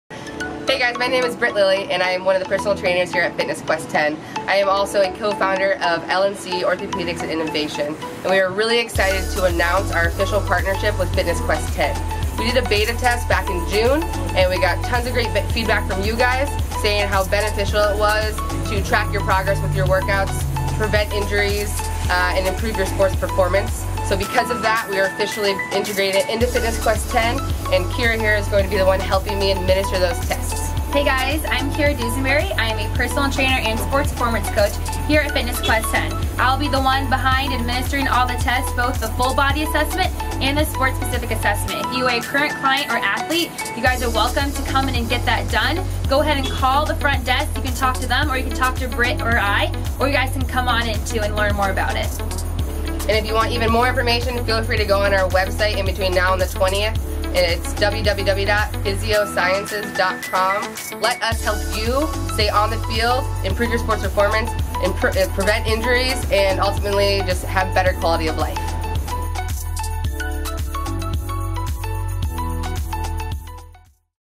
LCOI Assessments At Fitness Quest 10: Sports Specific & Full Body
Dr. Britt Lillie of L&C Orthopedics & Innovation announces full body and sports specific assessments available at Fitness Quest 10. Assessments are designed to help you improve your quality of life, move better, and perform your best.